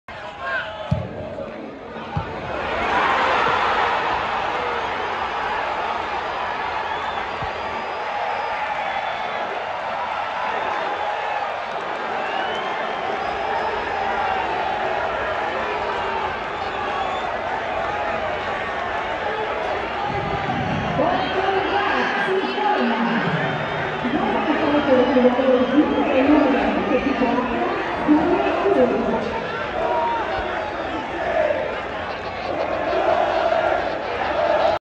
Gol Julio Furch | Atlético de San Luis 2-6 Atlas | Liga MX - Grita México - Jornada 15 | AtlasFC
EL EMPERADOR COLOCA EL TERCERO EN EL MARCADOR, CON UN GRAN CENTRO DE QUIÑONES!!! 🤩

LA JU-JU SIGUE HACIENDO GOLES!!! ⚽️